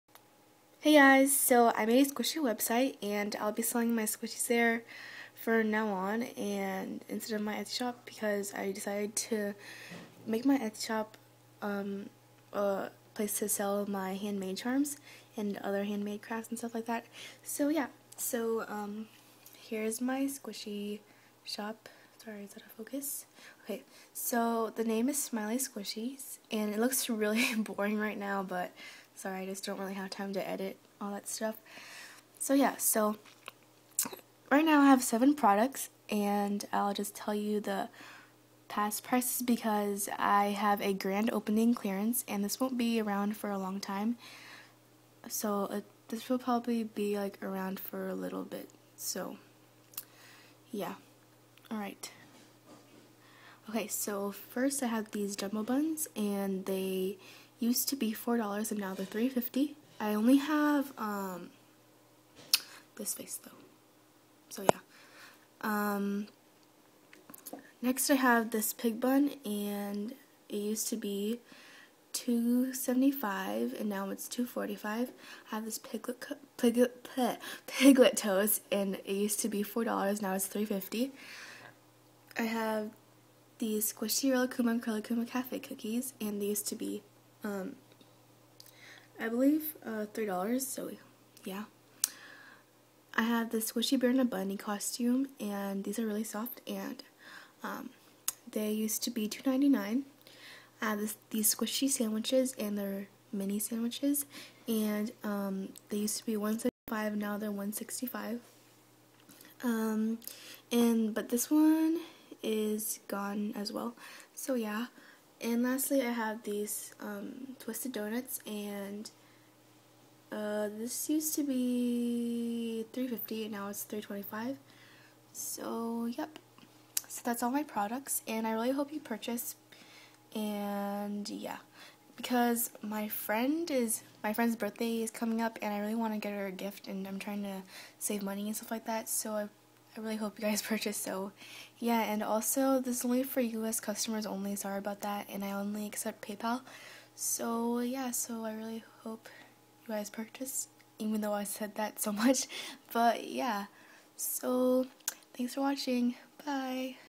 Squishy website! (SALE!)
PLEASE READ!

5th and 10th customer will recieve a free random squishy thats not in my shop!

I hope you purchase! Saving up for a gift for a very special, long time, best friend! The sale will end on May 13th! So purchase soon! :) thank you.